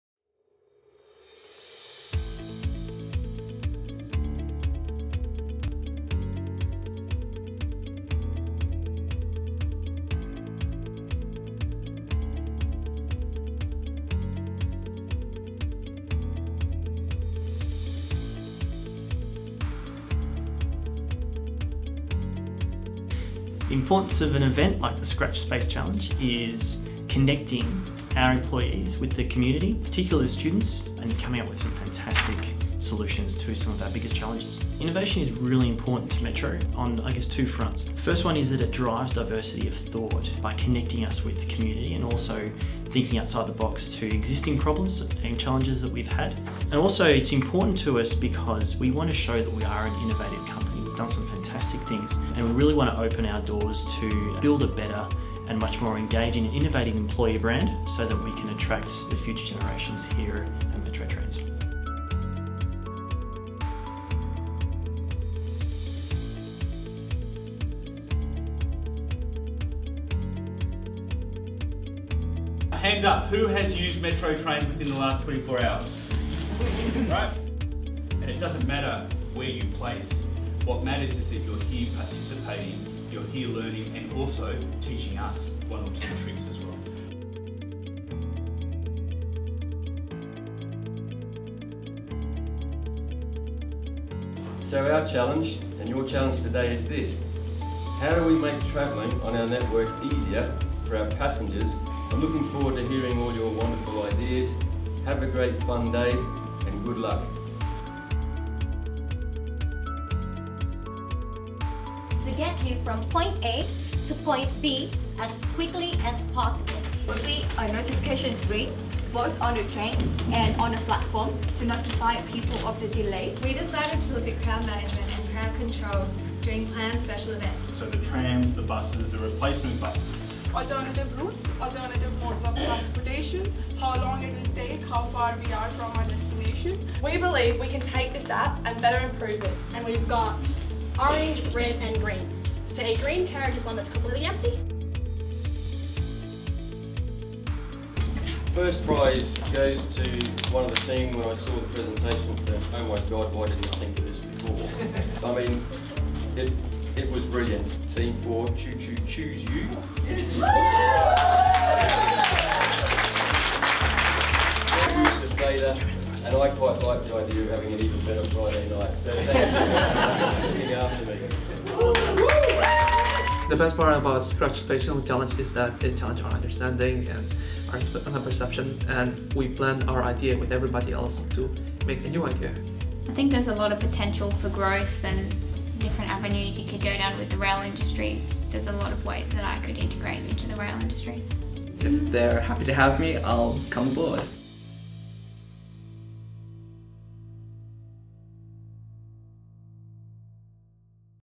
Paddl Games - Metro Trains Scratch Space Challenge
Client: Metro Trains Melbourne
Event: Scratch Space Challenge

Format: 1 Day Innovation Challenge

60 Student Participants
9 Universities
2 TAFE's
20 Metro Mentors
20 Metro Staff
10 Teams
18 Great Ideas
410 Coffees
180 Sandwiches

Outcome
The top 3 teams delivered brilliant and achievable concepts that have since been selected to progress to Business Case building. We can't wait to see where these innovative ideas go next!

To find out more about Paddl Games head over to paddljobs.com